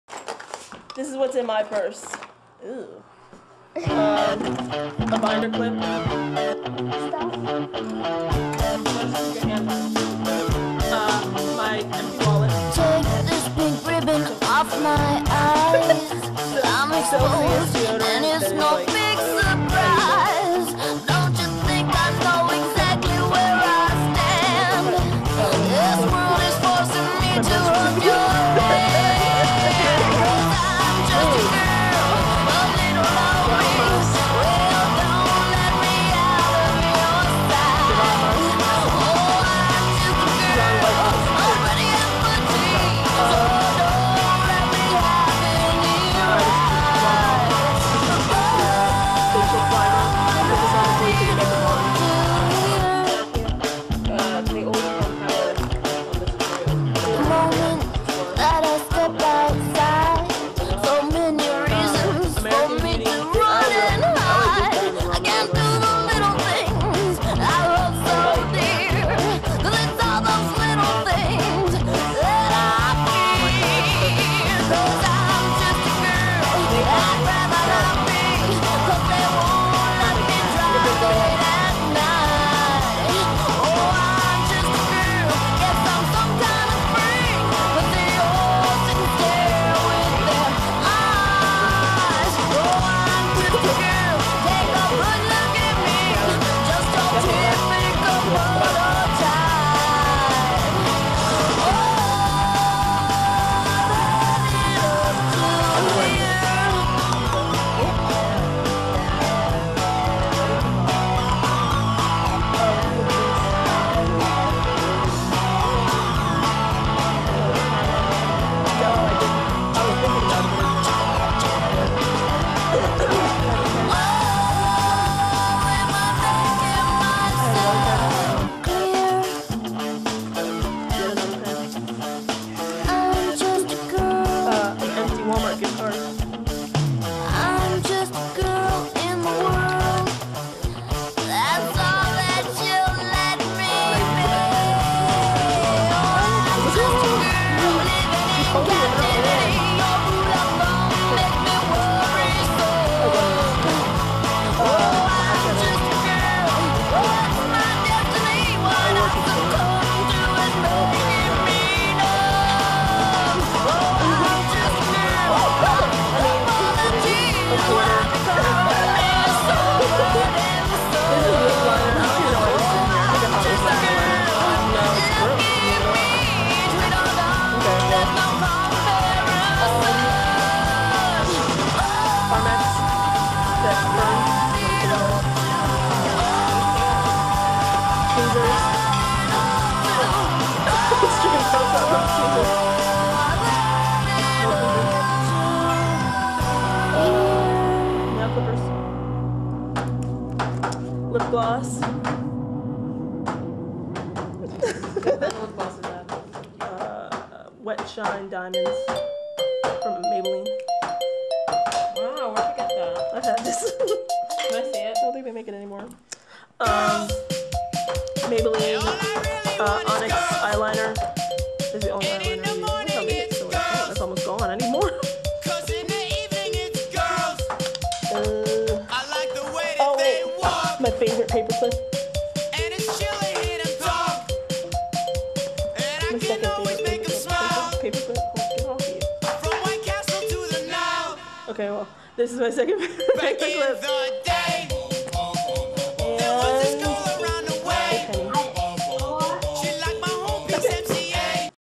Whose Purse Contents Are More Feminine
Shana and I were having a contest to see whose purst contents are more feminine.  Solana was the judge.